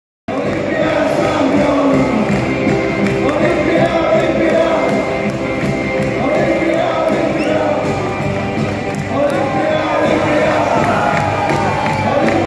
Albert Riera s poljubom po finalu pokala
Čustveni trener Olimpije Albert Riera v objemu svoje soproge.